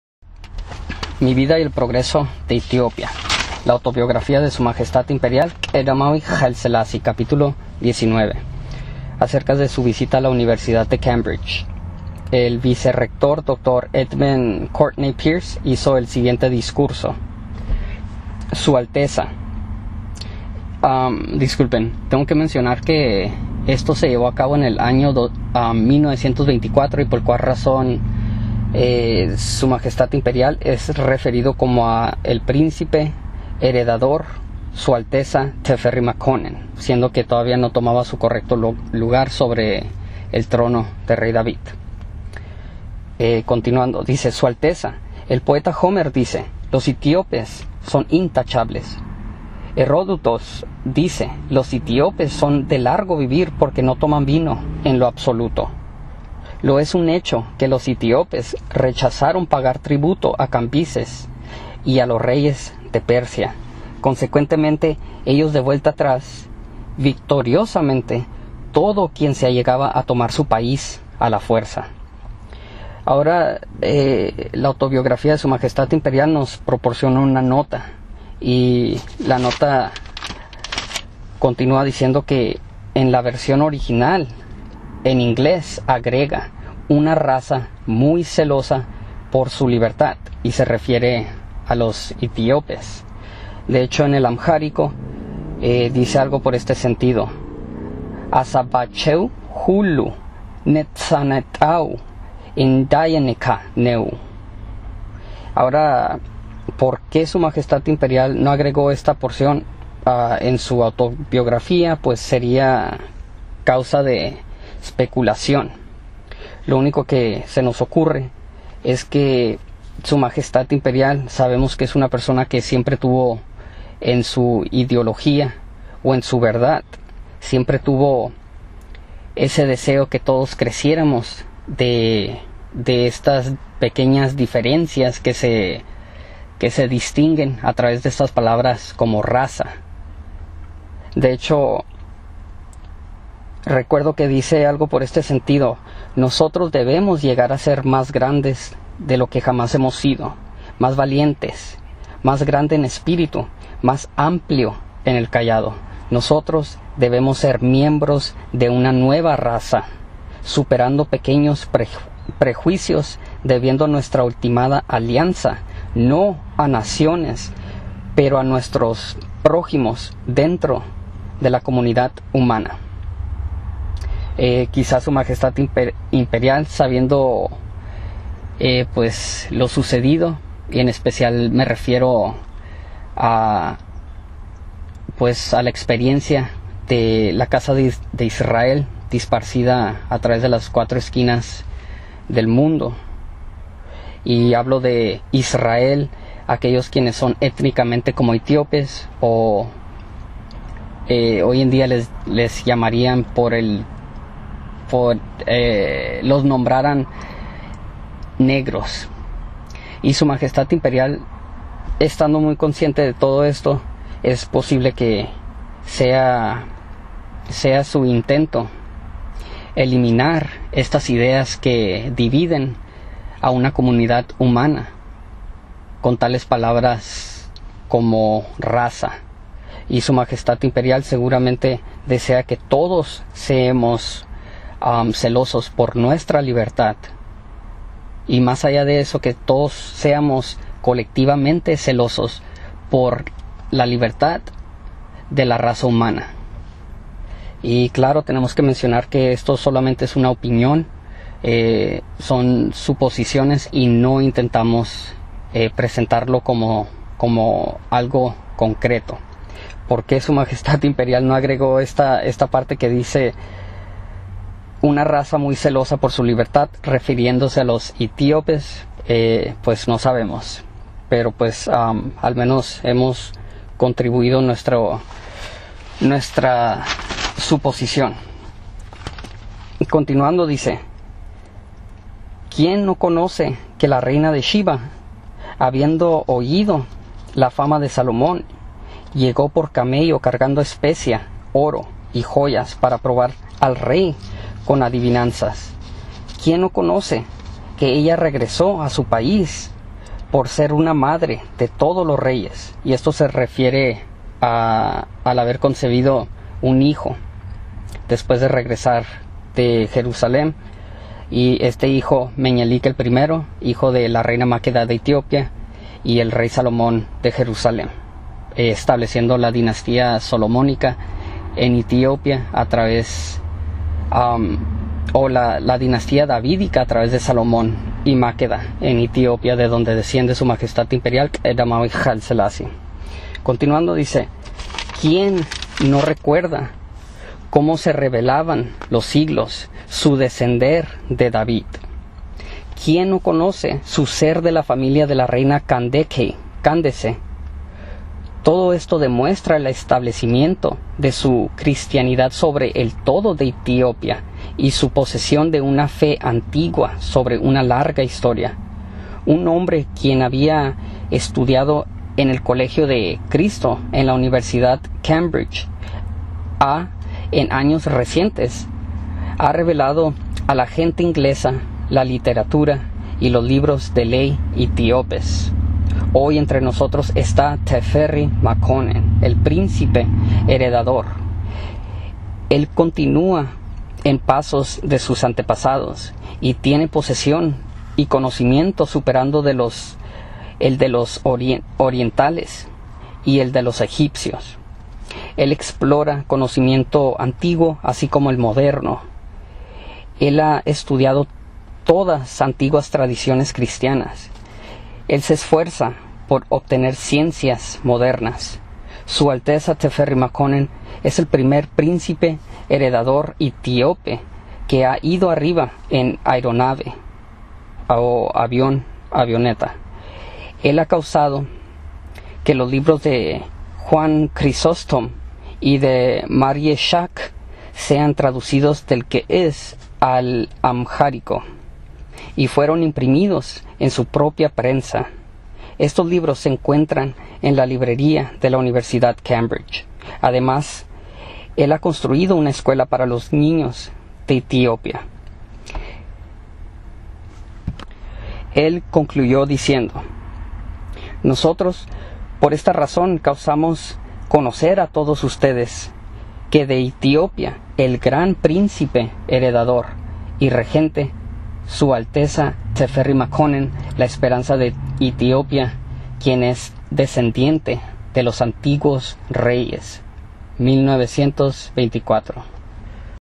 Universidad De Cambridge Recibe Su Altesa Tafari Makonnen Principe Heredador de Etiopia
"Mi Vida y el Progreso de Etiopia" Su Altesa Tafari Makonnen visita la Universidad Cambridge y el Vice Rector Dr. E. C. Pearce lo recibe con estas palabras...